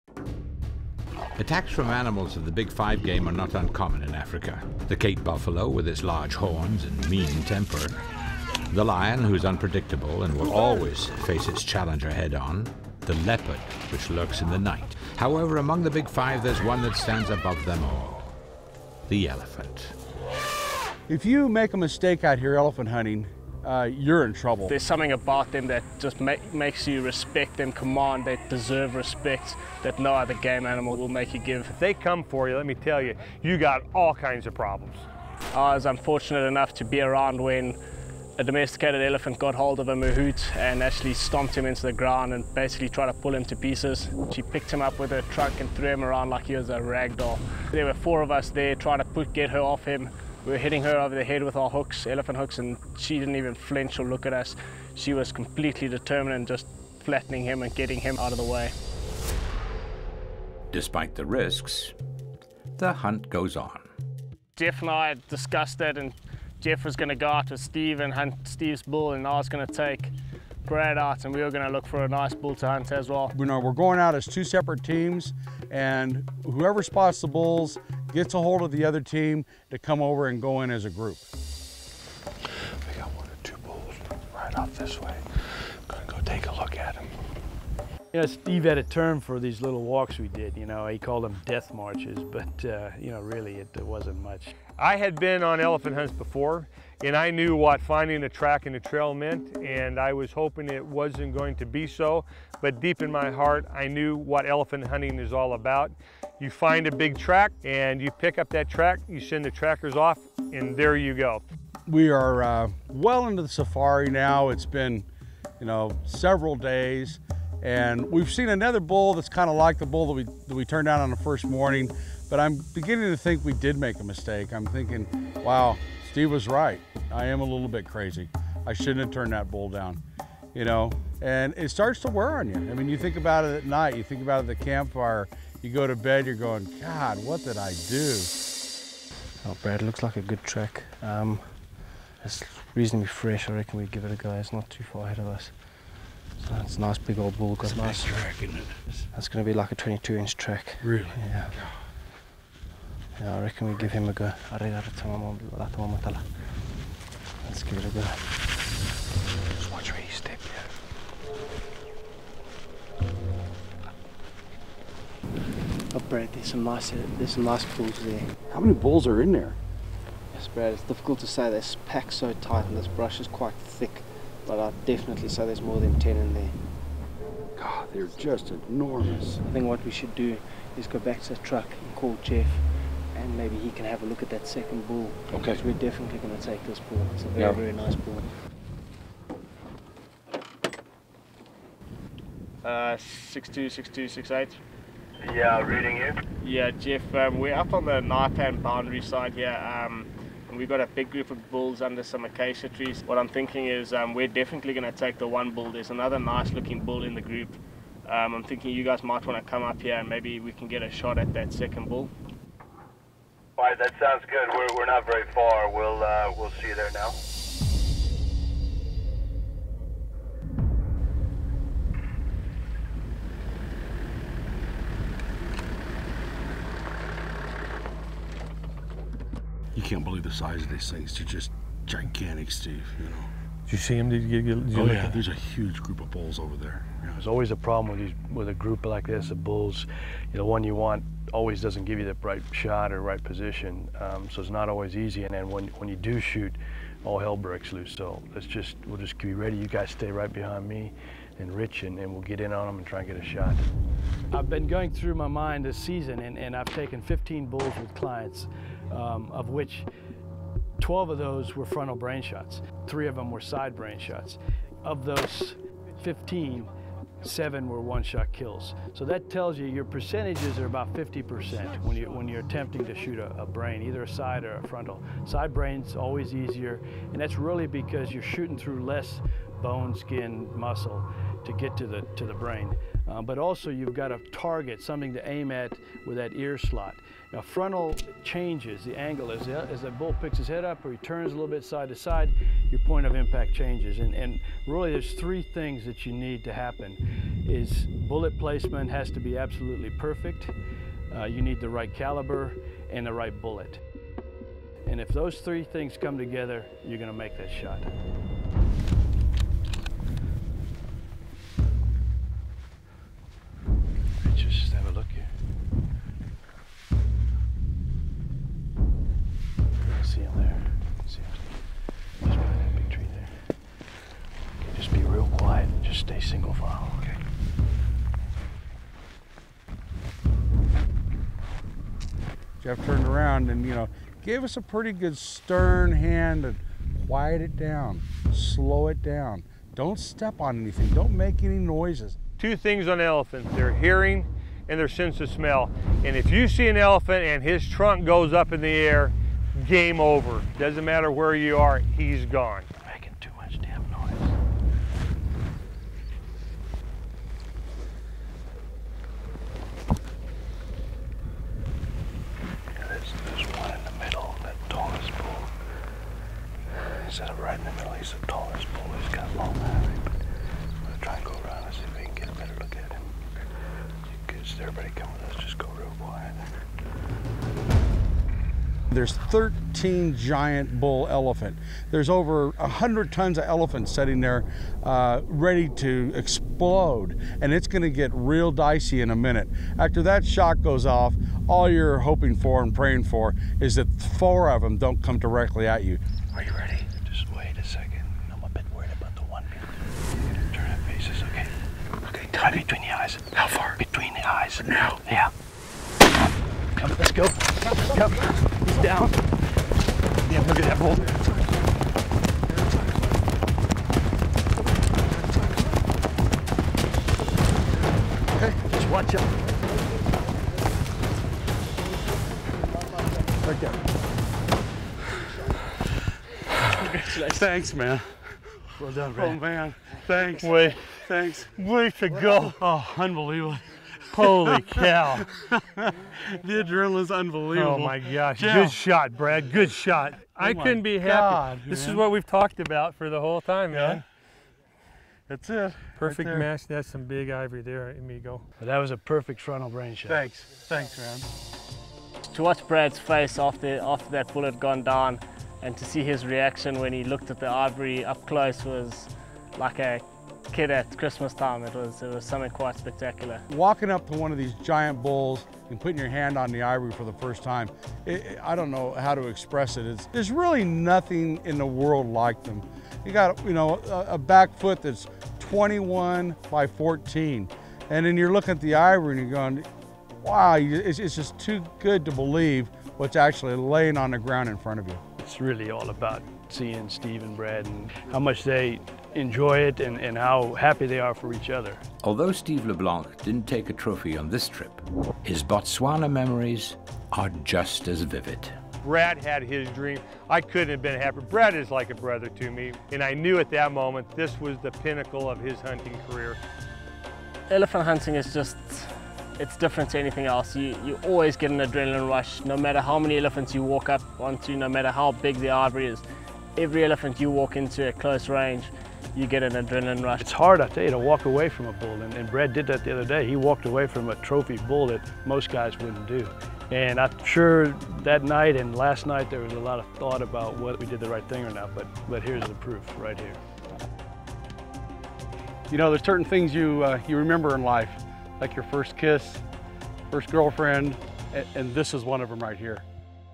Huge Botswana Bull for First-Time Elephant Hunter
An experienced hunter takes on the ultimate dangerous game challenge as he pursues his first elephant in a thrilling Botswana safari.